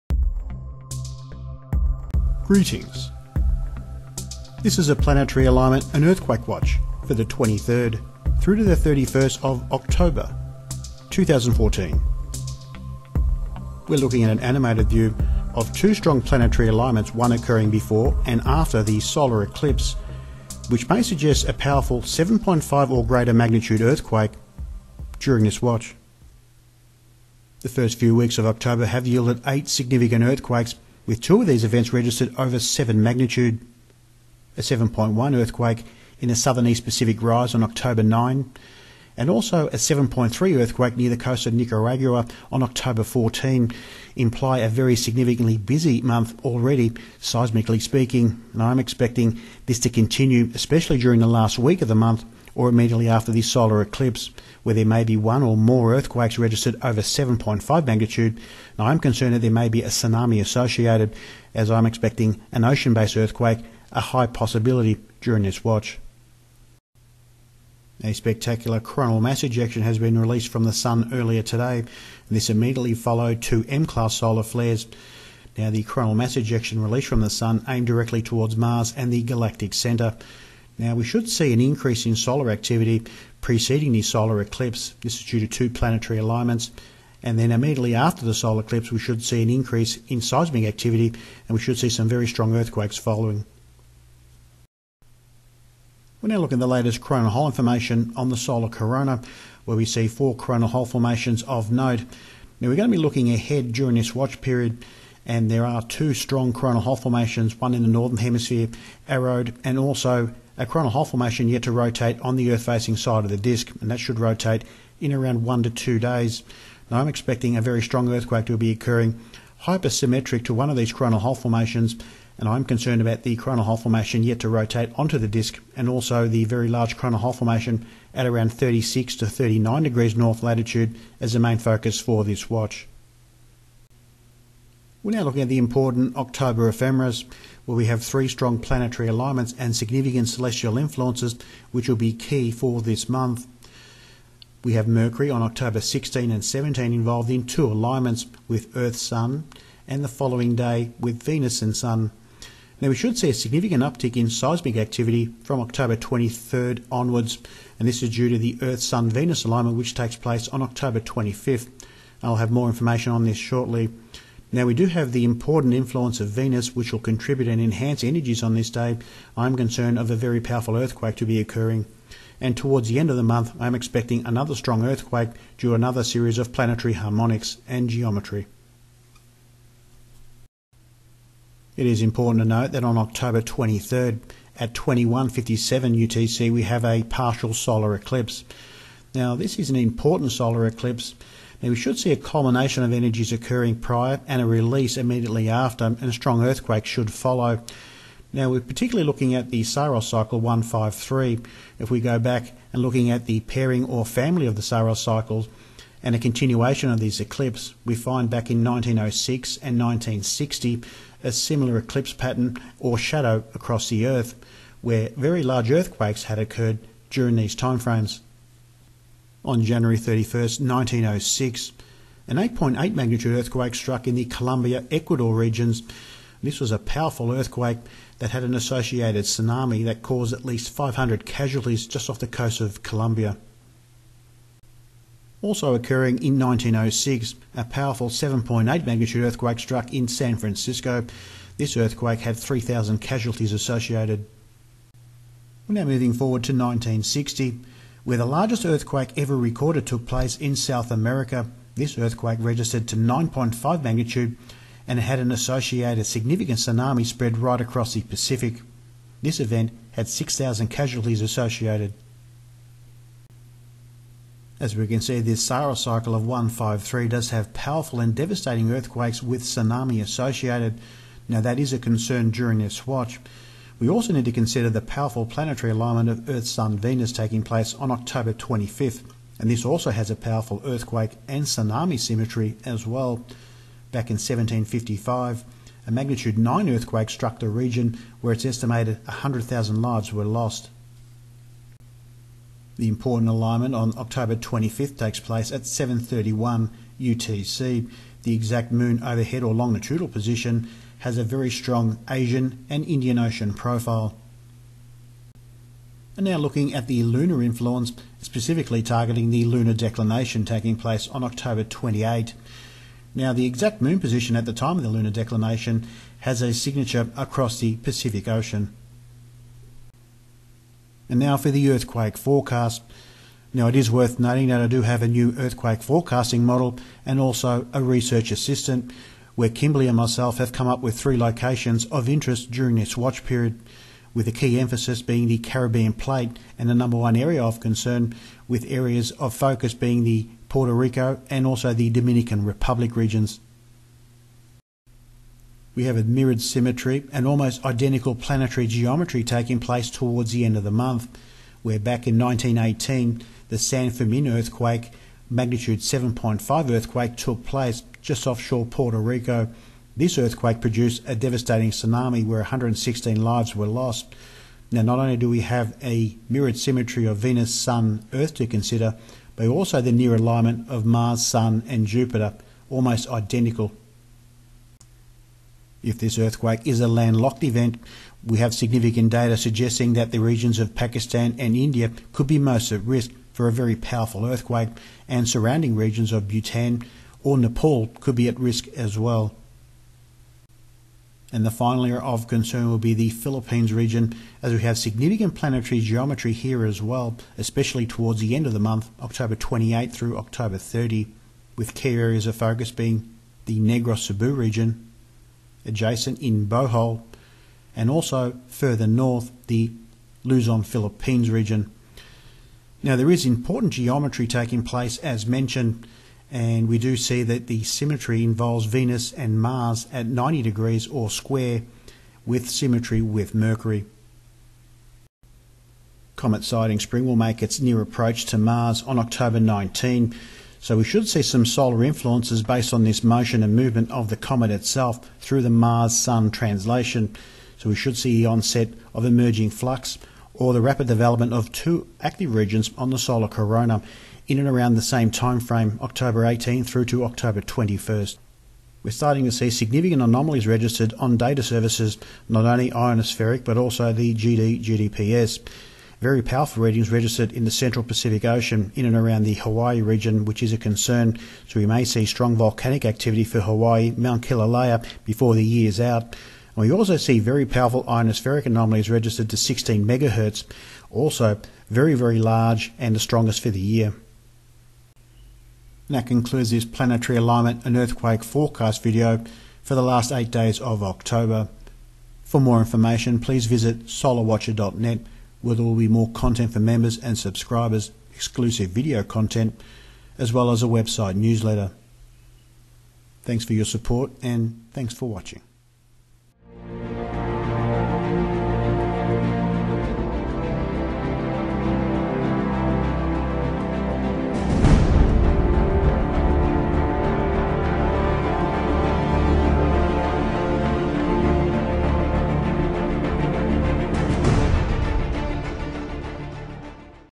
Planetary Alignment/Earthquake Watch Oct 23-31, 2014
A strong Planetary Alignment (Venus-Sun-Earth) on October 25 taking place after the partial solar eclipse is featured prominently this month may coincide with significant earthquake(s) over 7.5 Magnitude. Areas highlighted in this video are Philippines, Puerto Rico/Dominican Republic and the Pakistan/India Regions. If this event is Ocean based there may be a tsunami associated especially during October 23-25.

Google Earth
Updates and annotations will be added to the youtube video and description box.

The spinning world globe at the start of the video is a free program called Earthquake3D. This video may not be reproduced in whole or in part without express written permission.
Ending Music is composed and produced by Rick Horrocks

**Results Of This Watch**
M 7.1 - FIJI REGION - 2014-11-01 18:57:22 UTC
M 6.0 - EASTER ISLAND REGION - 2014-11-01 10:59:58 UTC
M 6.1 - TONGA - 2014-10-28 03:15:38 UTC